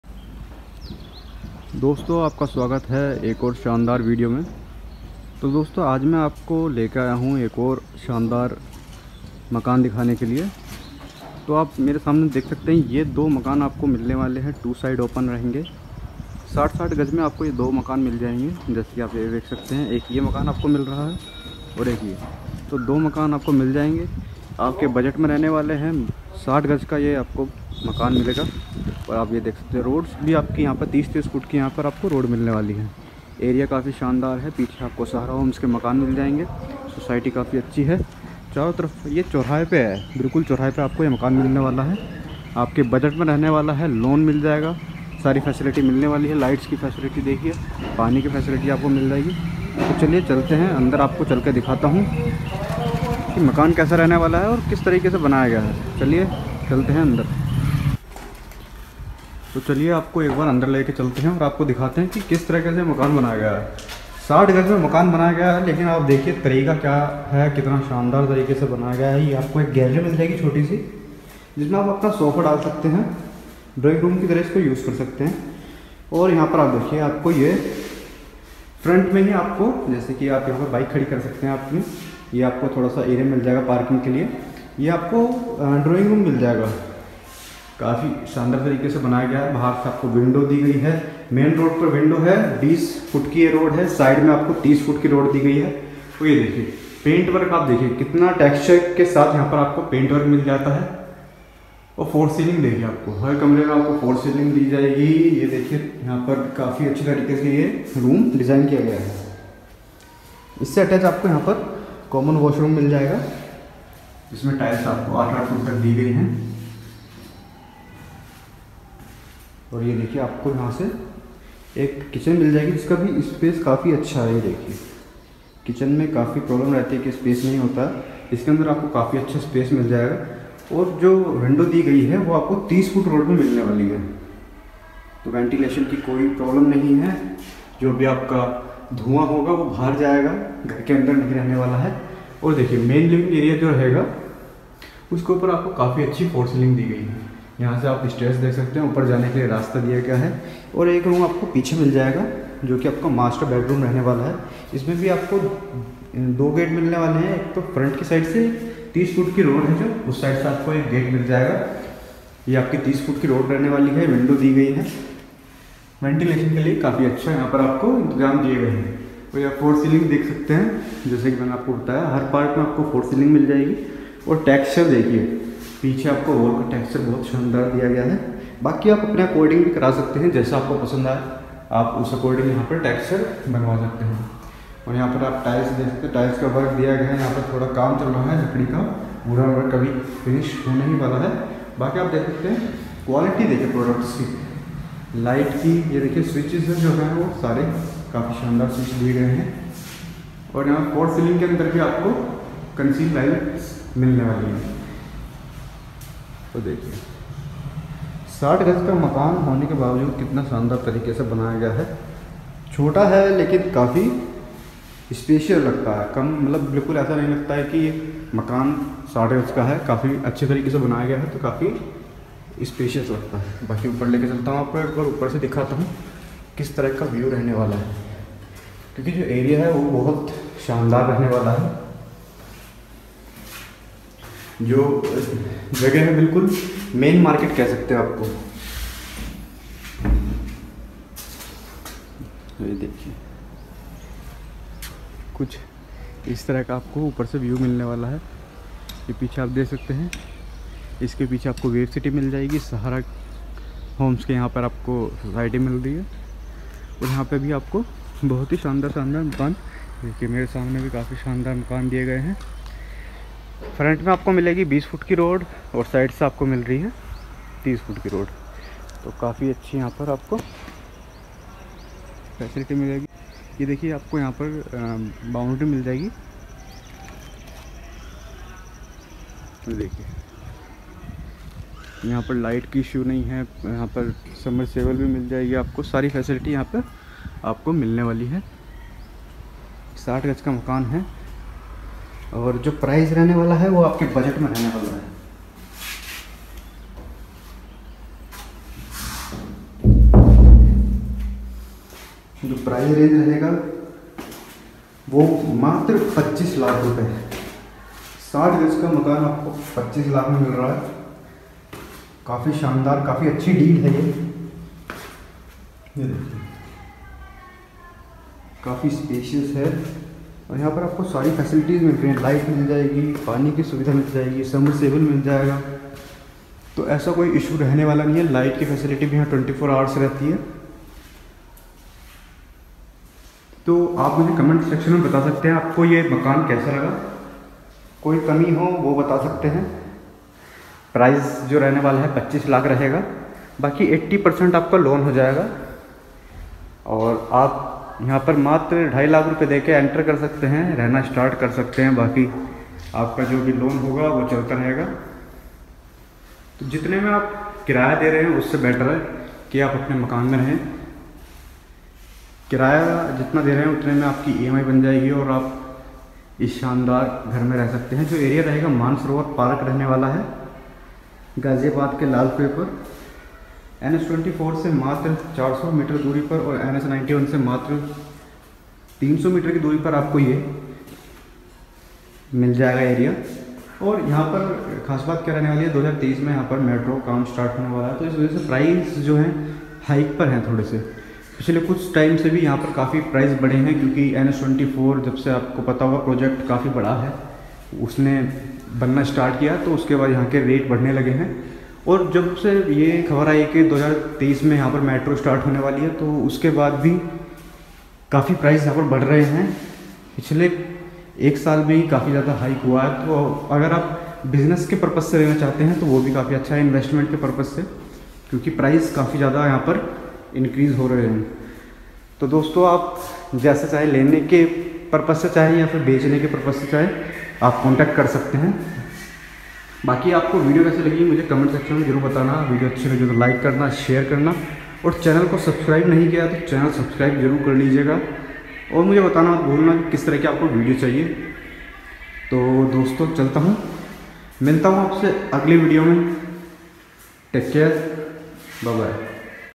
60 गज़ में बना बनाया मकान मानसरोवर पार्क ग़ाज़ियाबाद में | Individual house in Ghaziabad
(अगर आपको हमारी videos अच्छी लगती है तो हमे support करे Phonepe :- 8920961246)

Ring light
Ring light

In this video i am showing you 60 yards (gaj) Independent House